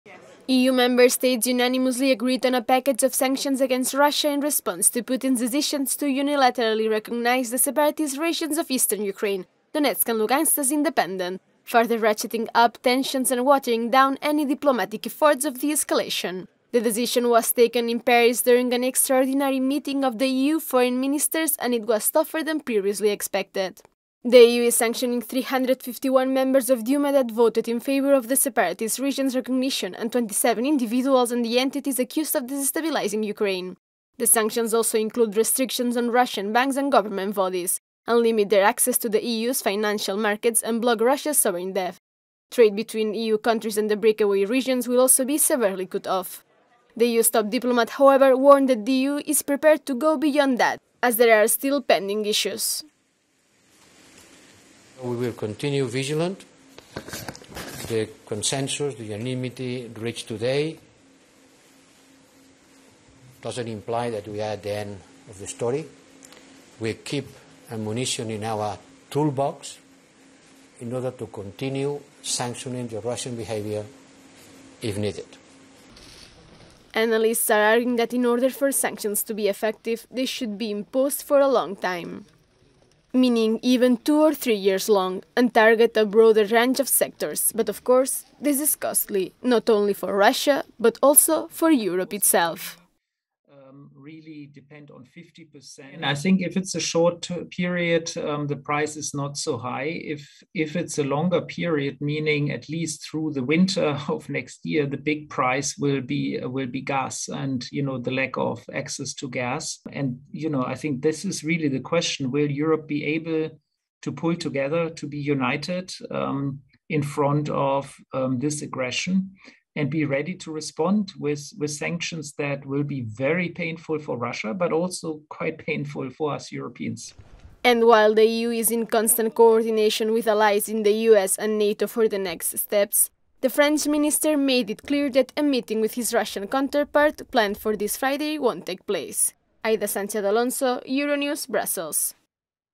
EU sanctions 351 Russian lawmakers who voted to recognise Ukraine's separatist republics
The fresh package of sanctions also limits Russia's access to the EU capital and financial markets.